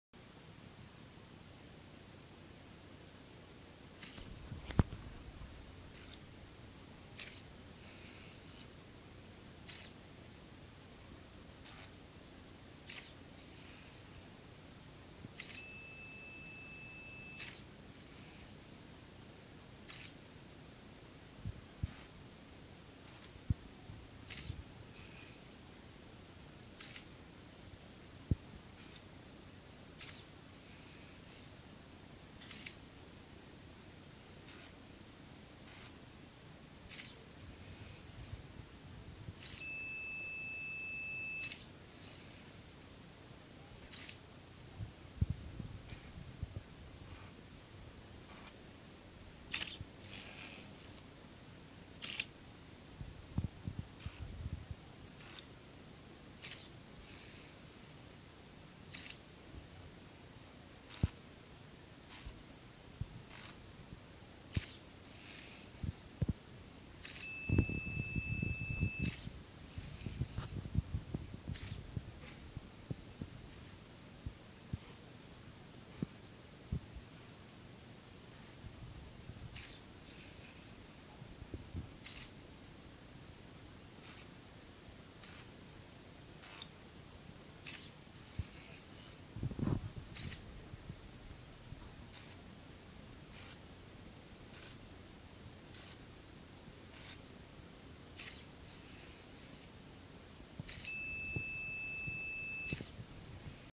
The FIBIAC - 3D-printed electromechanical computer calculating the fibonacci sequence.
My prototype electromechanical computer calculating the fibonacci sequence. Results appear in the register on the right, and the alert-light signals a new result is ready. The program uses 12 punch cards. Most of the machine was printed on my Makerbot thing-o-matic